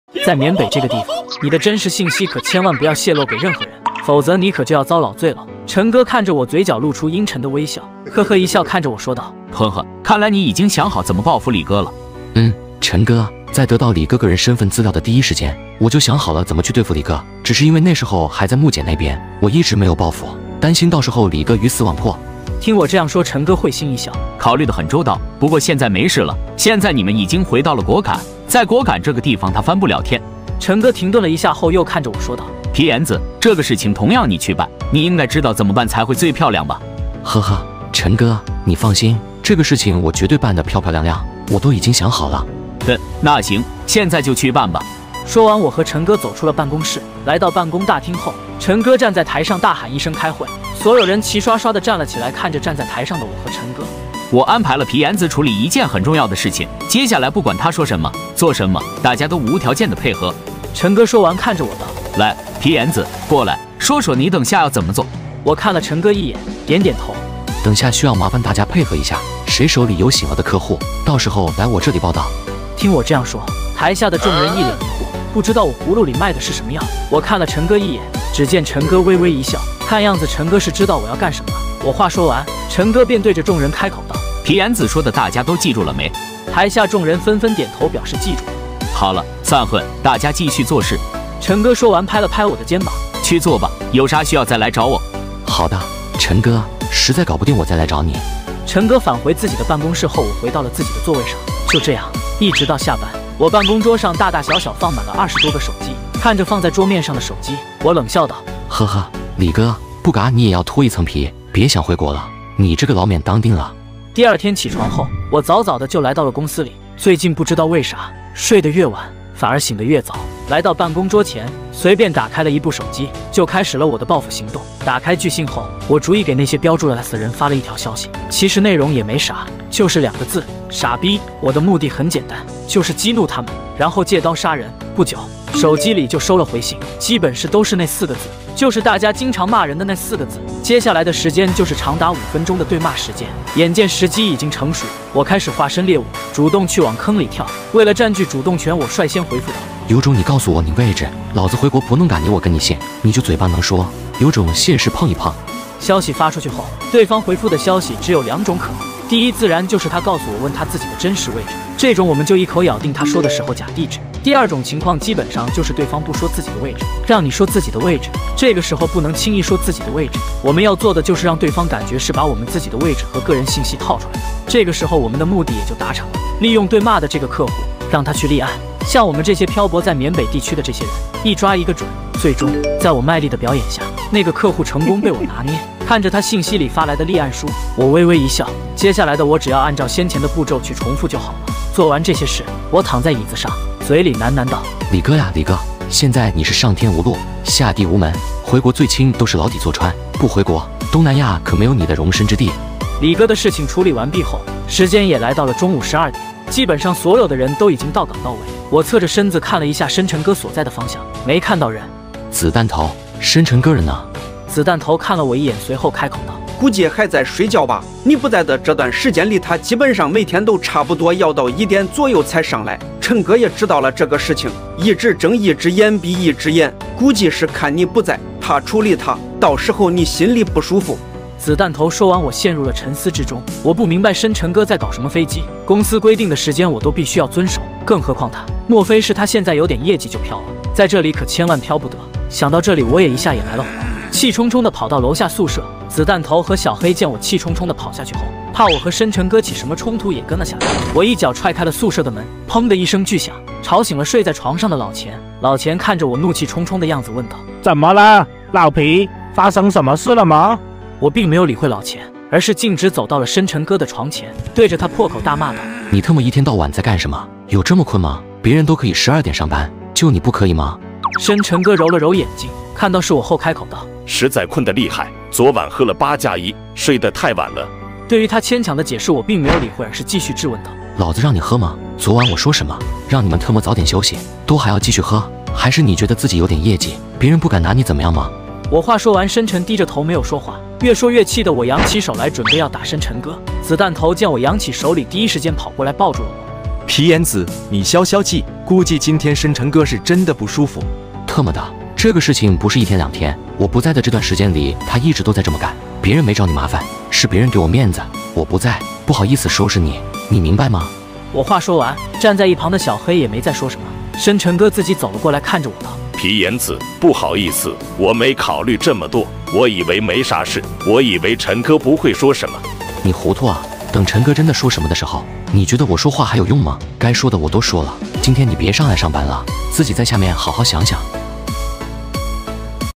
緬北那些年EP97｜ 瘋狂報復李哥，飄了的深沉哥，考慮周全準備實施【皮炎紫喲】
本頻道製作的所有視頻都是原創拍攝的短劇，絕對符合平臺的使用策。 哈嘍，大家好
我是【皮炎紫喲】
這是我在YouTube唯一賬號
主要以動畫形式向大家分享一些日常